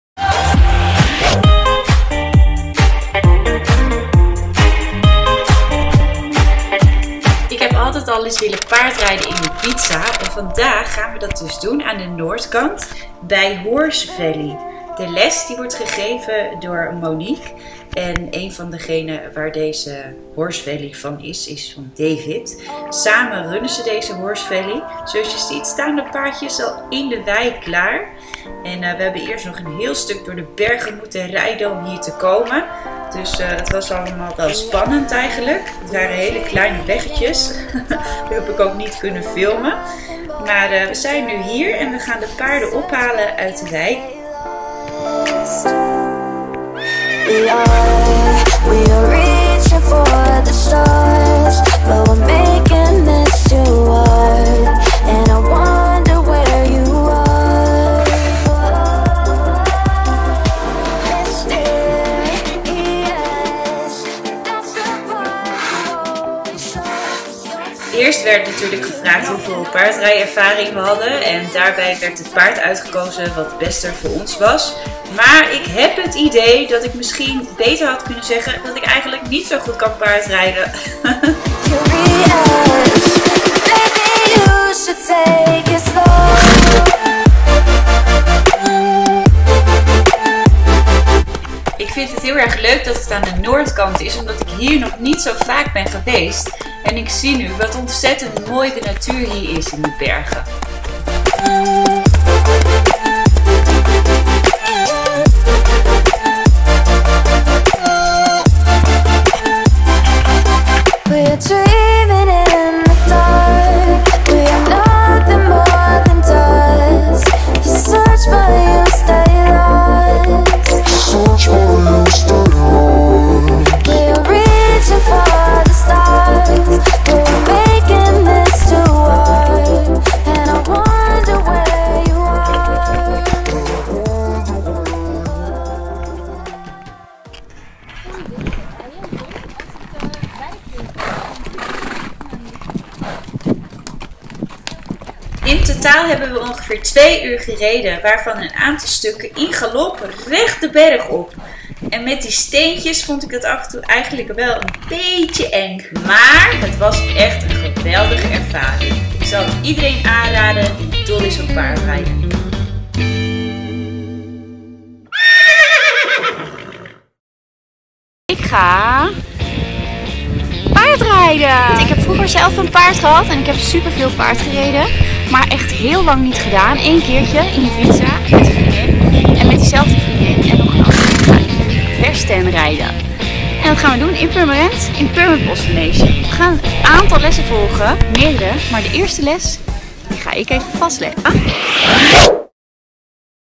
PAARDRIJDEN OP IBIZA - HORSE VALLEY - diesnaloomans.nl
Want elke maandag komt er een nieuwe video online! :

sorry dat deze video wat schokkerig is, ik heb nogal moeite om hem goed vast te houden tijdens hert rijden:) Volgende keer maar weer met harnas om!

SOCIAL LINKS!

VOOR SAMENWERKINGEN:
♥ Diesna Loomans ♥

♥ FITNESS-FOODNESS-HAPPINESS